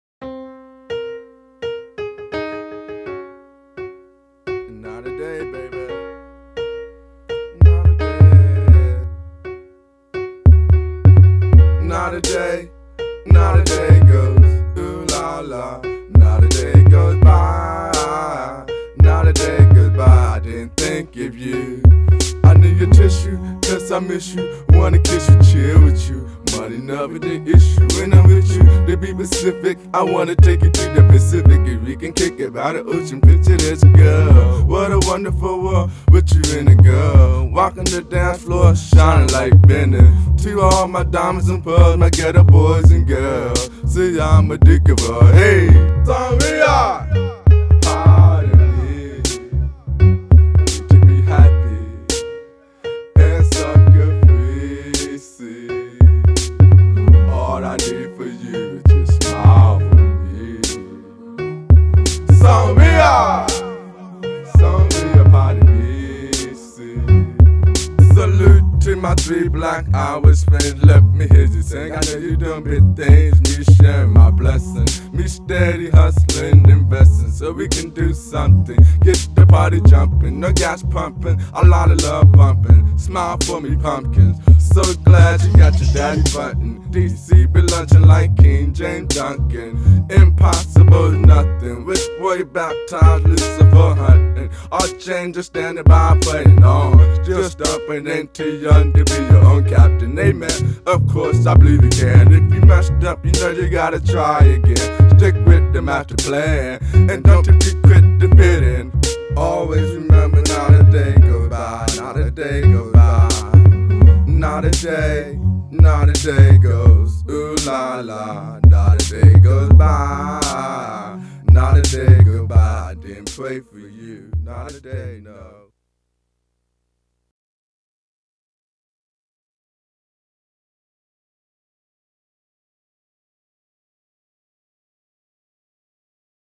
Not a Day
This video is dedicated to anybody who is missing someone in their life. This could be your mother, your loved one, or a child. Sonria!

--Def Nation Royals